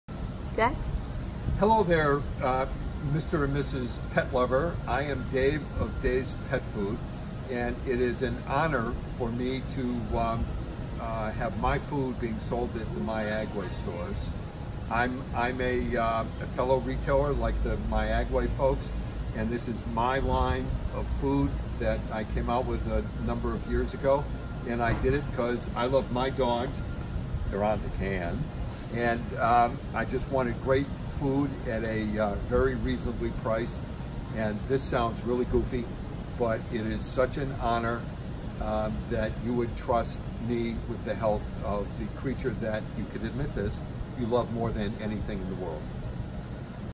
Introducing Dave Ratner of Dave's Pet Food
Shot on location at myAGWAY of North Branford. Dave Ratner is proud to announce his new partnership with myAGWAY, who has introduced his line of premium canned pet food.